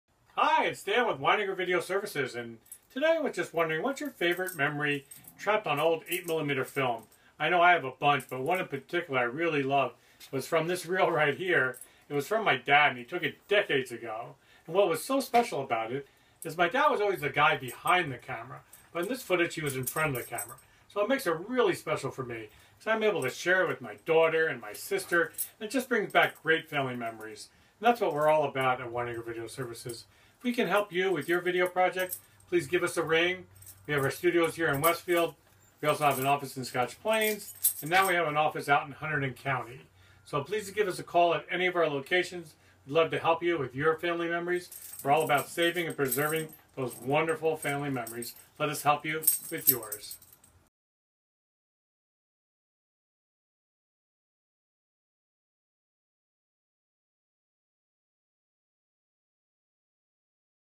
What is your best family memory trapped on old 8mm film?
What is your best family memory trapped on old 8mm film? At WeinigerVideo Services we specialize in 8mm and 16mm to DVD as well as VHS to DVD and many other services.